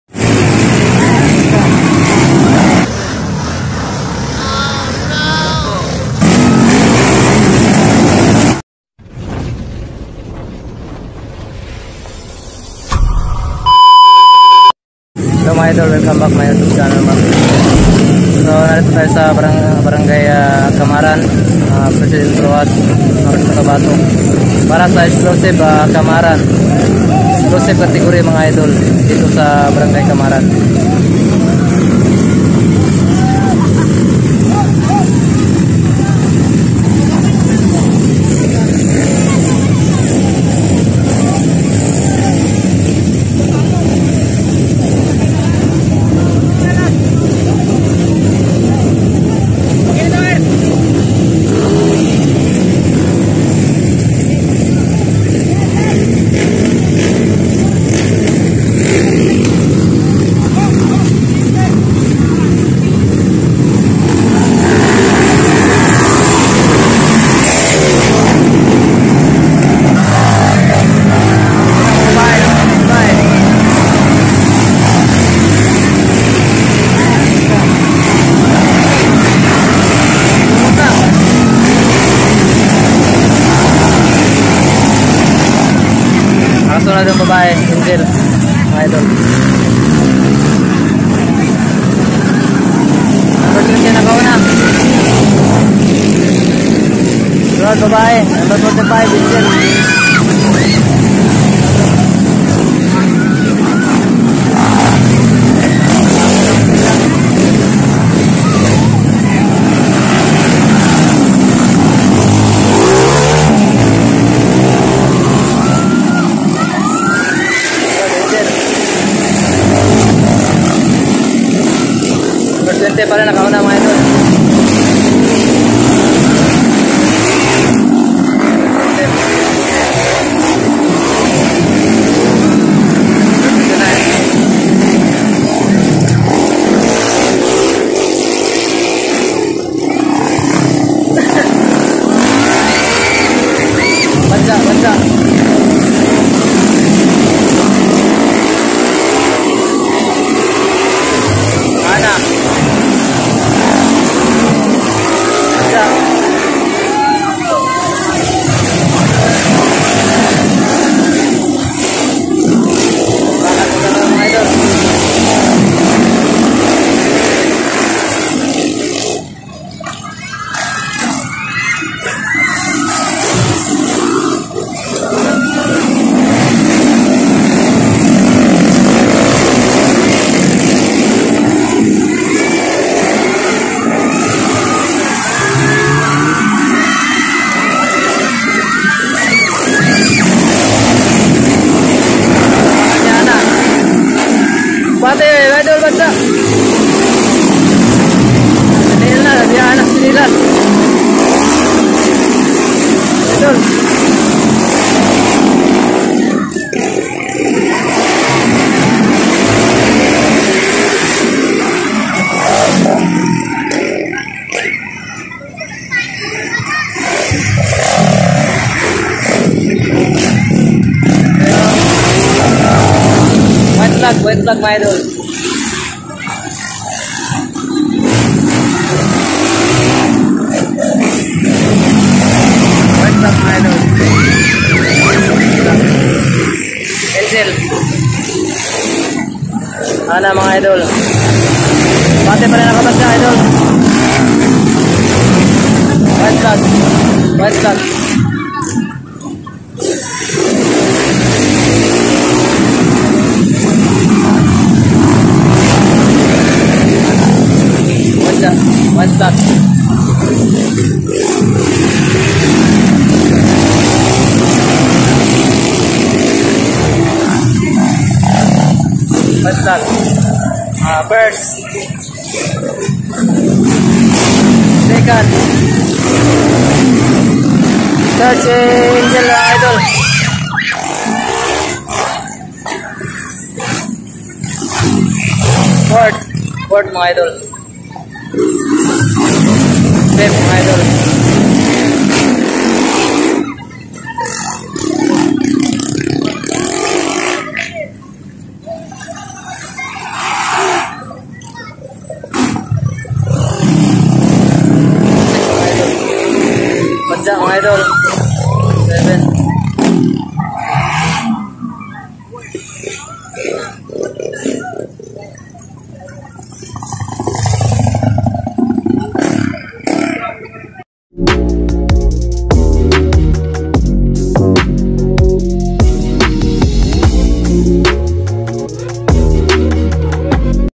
NAIPIT AKO SA PALIKO | BAJA100 POWER PALA | EXCLUSIVE CATEGORY BRGY.KAMARAHAN
Kalaban ng baja100 mga xrm at klx | Exclusive Category Brgy. Kamarahan

64th Anniversary Brgy. Kamarahan President Roxas North Cotabato

January 20,2023

Please follow facebook PA share pa Follow sa ating fb